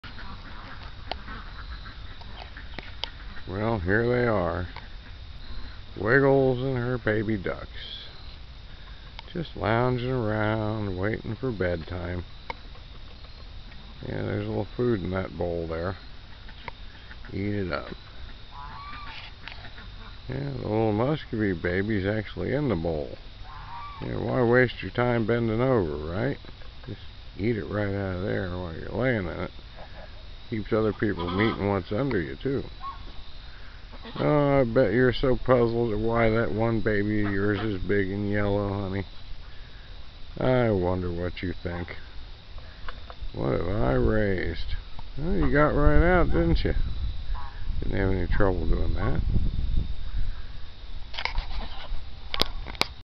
October Baby Ducks
Wiggles has two babies. One is a Muscovy like she is but the other is a baby Pekin duck. It is big and yellow and Mom seems confused at her baby being so big already.